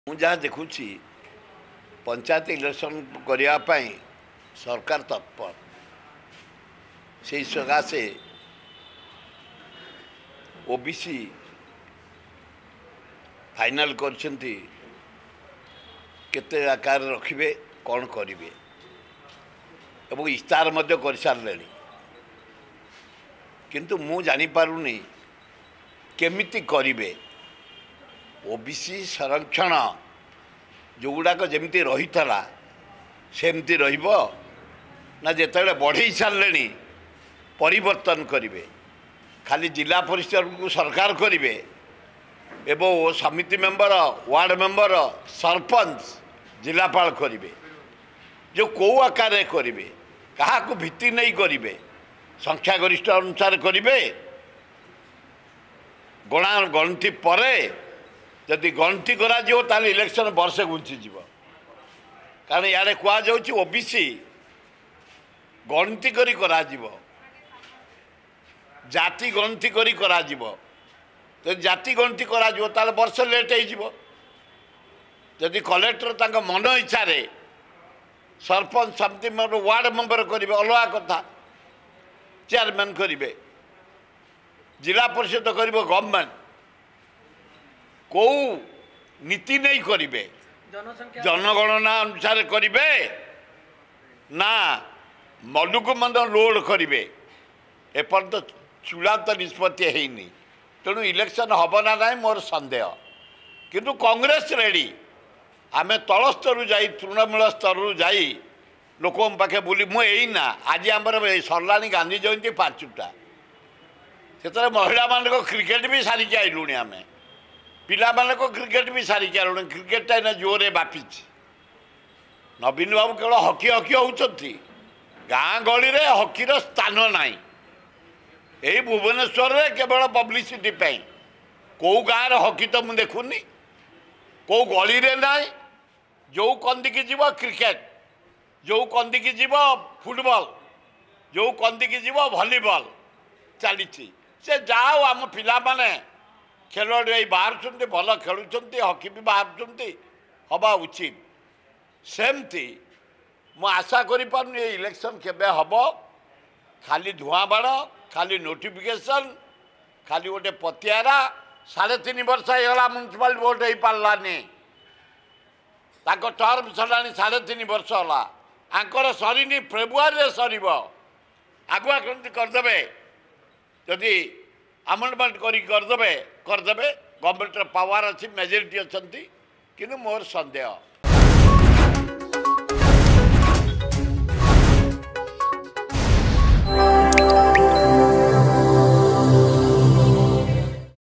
ଇଲେକ୍ସନ ହବ ନା ନାହିଁ ମୋର ସନ୍ଦେହ,କିନ୍ତୁ କଂଗ୍ରେସ ରେଡ୍ଡୀ : ସୁର ରାଉତରାୟ,କଂଗ୍ରେସ ବିଧାୟକ
ଇଲେକ୍ସନ ହବ ନା ନାହିଁ ମୋର ସନ୍ଦେହ,କିନ୍ତୁ ଆମେ ରେଡ୍ଡୀ  : ସୁର ରାଉତରାୟ,କଂଗ୍ରେସ ବିଧାୟକ